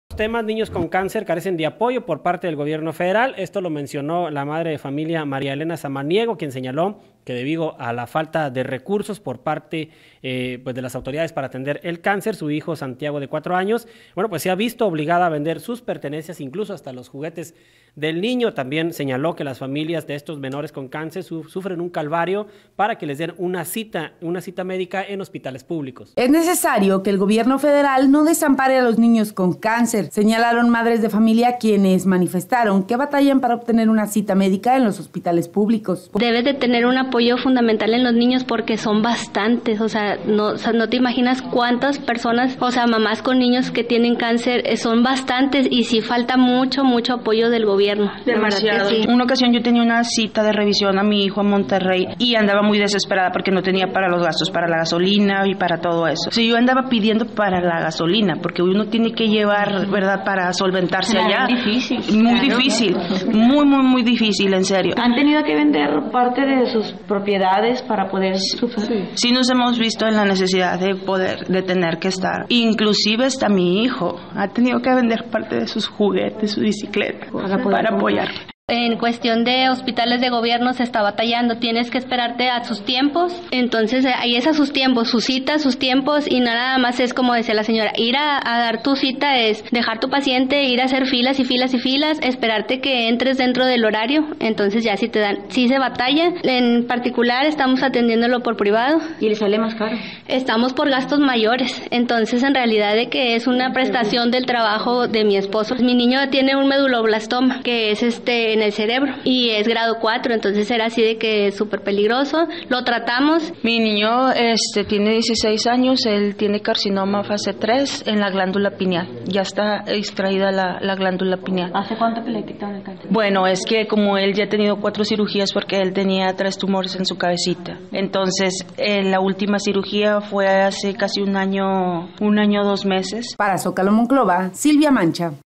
Monclova Niños con cáncer carecen de apoyo por parte del Gobierno Federa.
Monclova Niños con cáncer carecen de apoyo por parte del Gobierno Federal, mencionó la madre de familia María Elena Samaniego, quien señaló que debido a la falta de recursos para atender el cáncer de su hijo Santiago de 4 años, se ha visto obligada a vender sus pertenencias e incluso han tendido que vender los juguetes del pequeño.